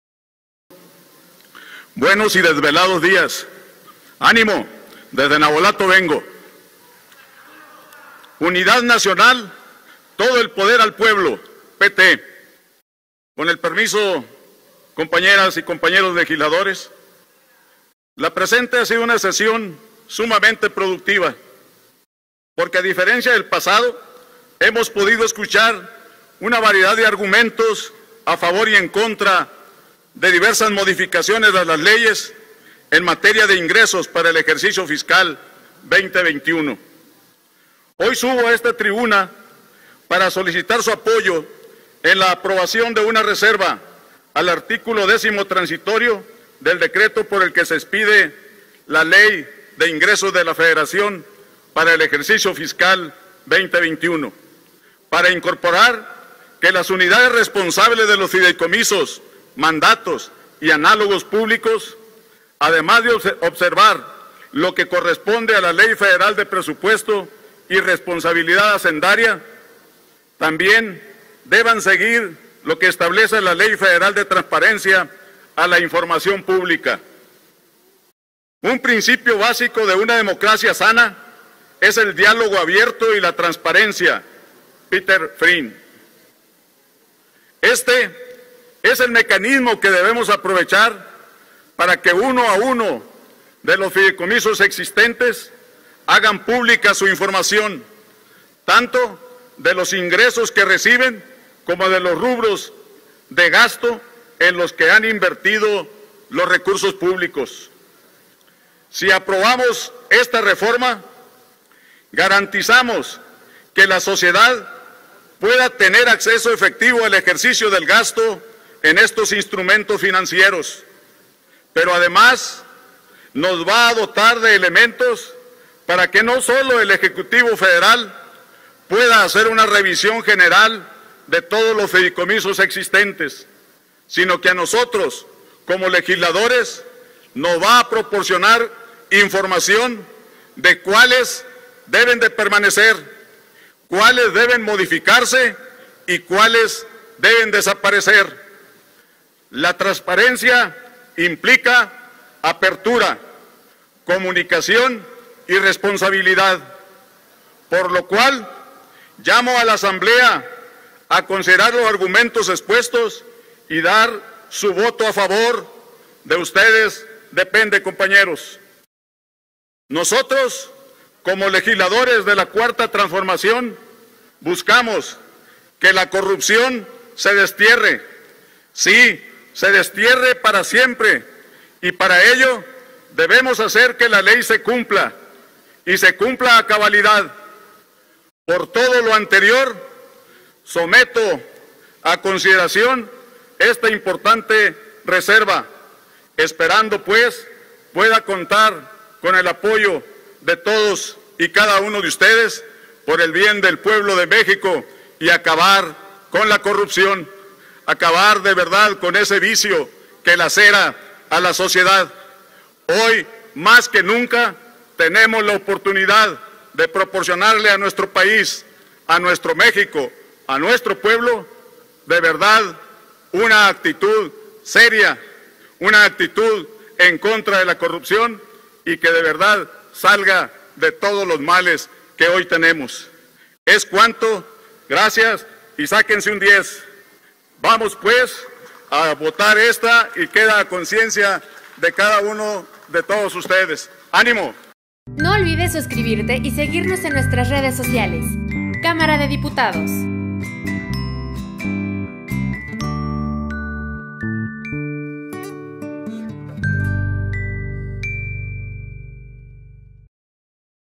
Dip. Jesús Fernando García Hernández (PT) / Ley de Ingresos de la Federación
El diputado Jesús Fernando García Hernández, del PT, presenta su reserva al art. Décimo Transitorio del dictamen por el que se expide la Ley de Ingresos de la Federación para el Ejercicio Fiscal de 2021.